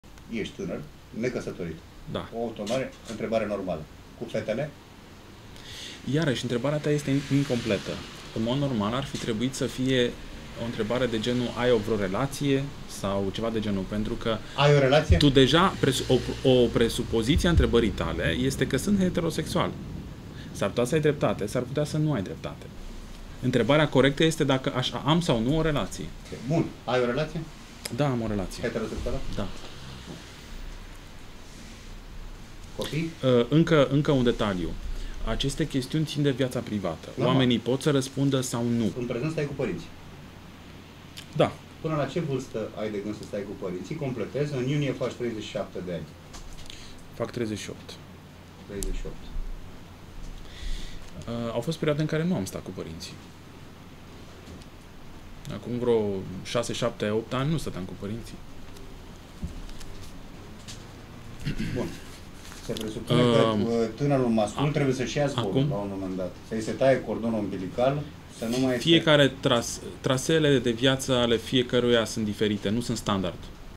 Remus Cernea: E gresit sa presupui ca sunt heterosexual...
Consilierul Primului Ministru, Remus Cernea: românii, un popor sexist, rasist, homofob și fundamentalist